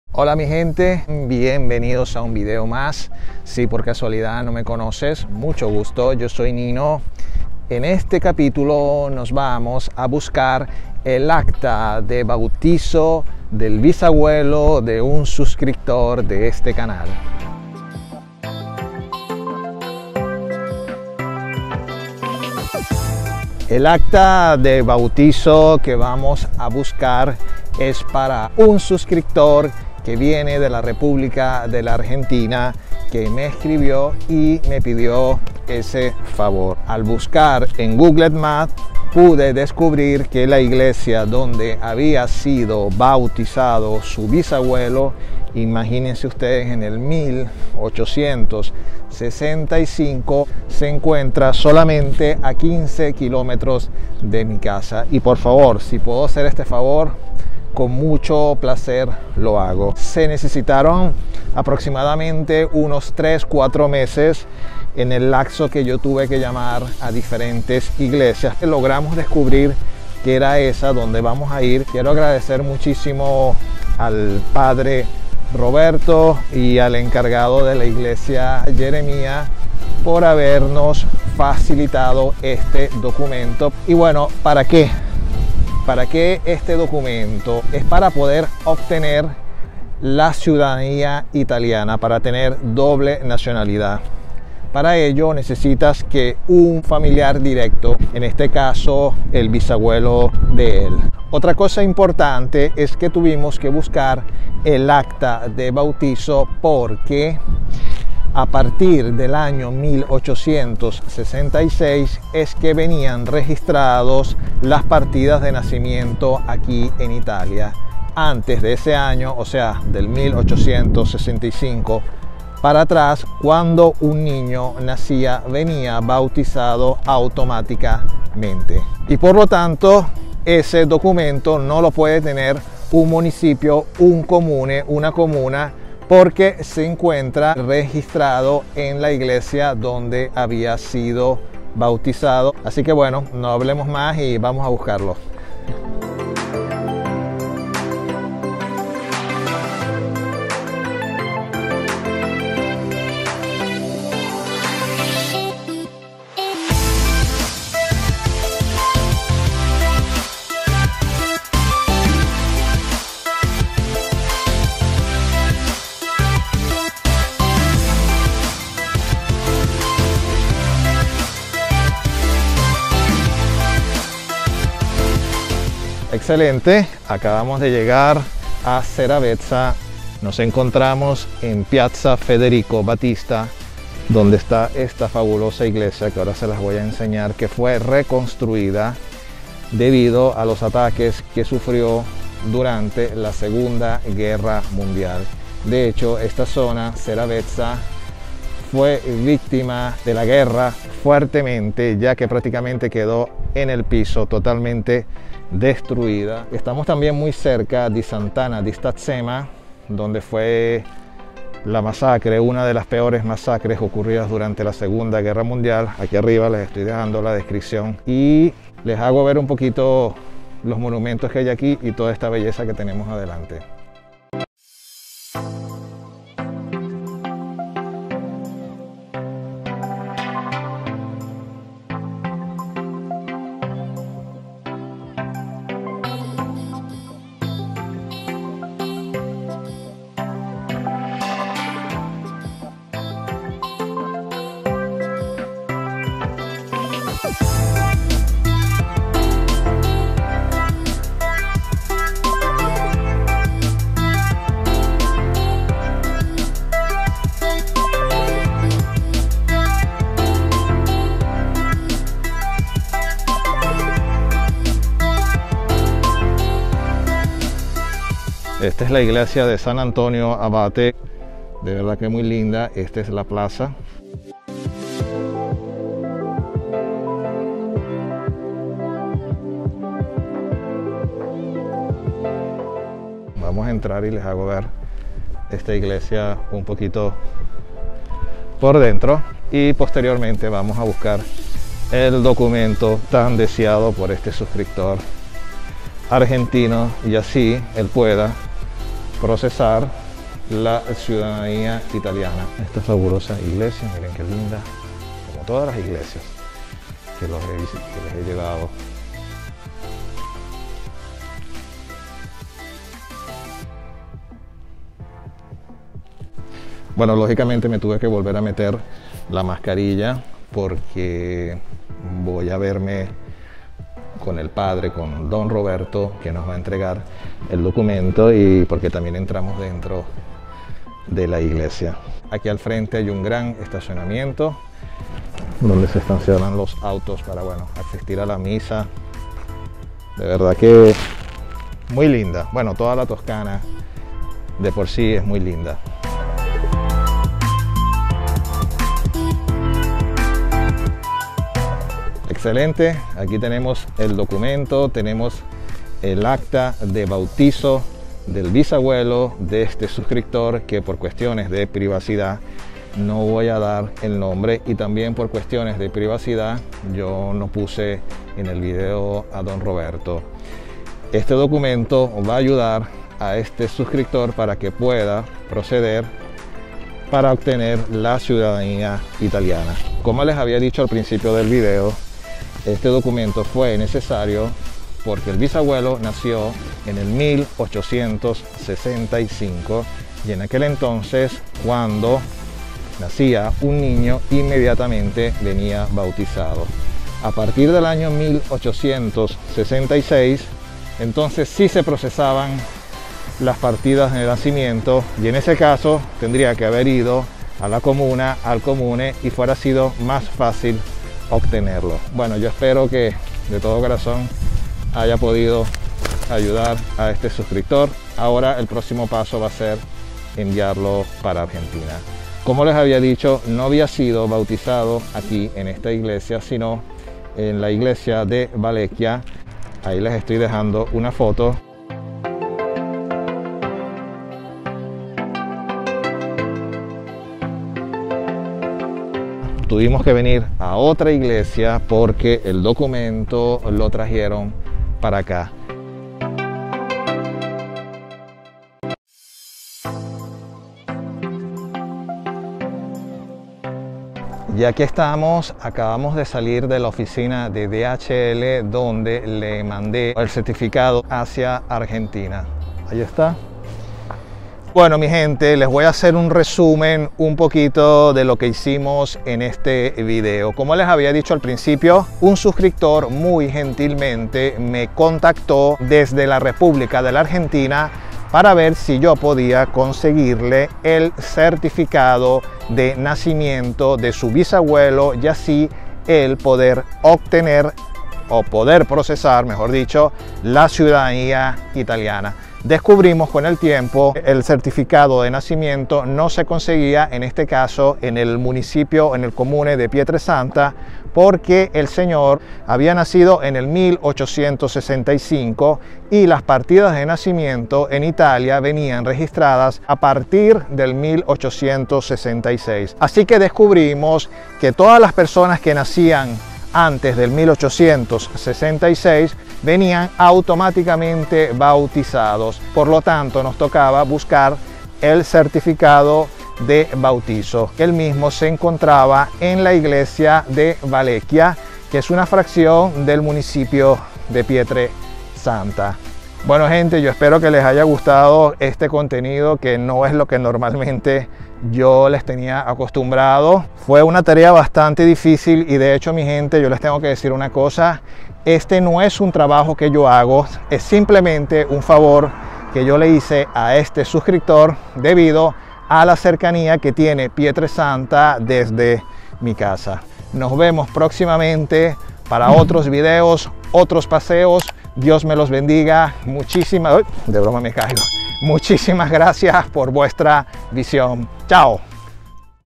Ayudando a un suscriptor a obtener la ciudadanía Italiana (primer paso)Italia
Nota: este no es un trabajo que yo hago,es simplemente un favor que hice para un suscriptor debido a que el lugar donde fui está solamente a 15 Kilómetros de mi casa.

Me pueden seguir también en Instagram @bolnino